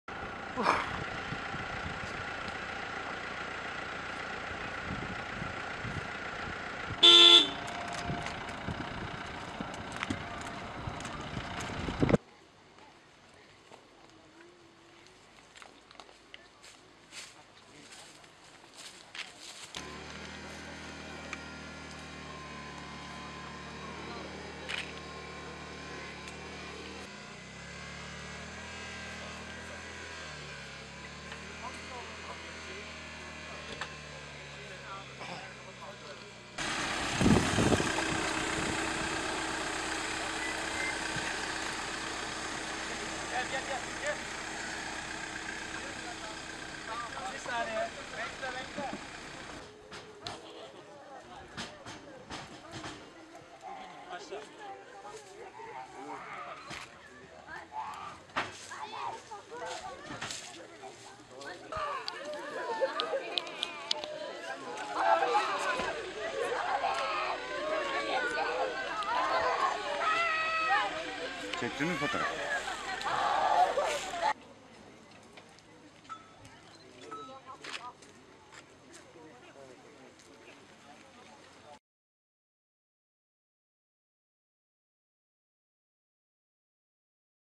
Yürekleri yakan kazada baba oğul can verdi
Manisa'nın Soma ilçesinde oğlunun da üzerinde bulunduğu traktörle fasulye dövmeye çalışan şahıs, ters dönen traktörün altında kalarak 5 yaşındaki oğluyla birlikte can verdi.